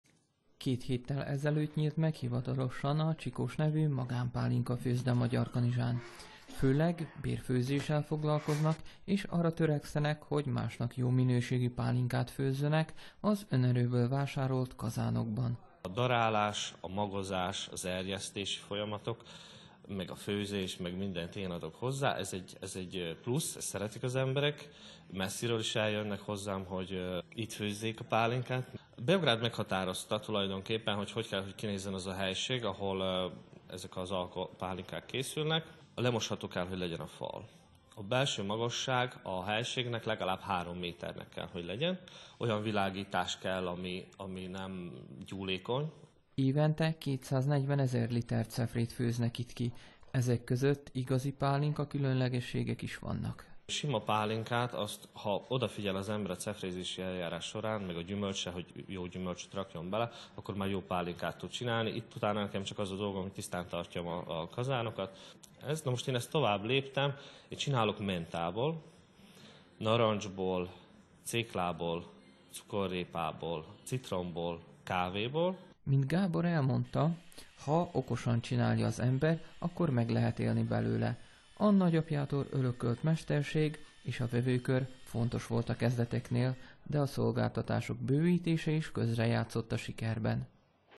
Pálinka készül kávéból, céklából, citromból Magyarkanizsán
Már javában zajlik a pálinkafőzés Vajdaság-szerte, habár a késő őszi termések lepárlása most van folyamatban. Az erre vonatkozó törvények pontosan meghatározzák, milyen keretek között lehet ma Szerbiában pálinkafőzdét üzemeltetni.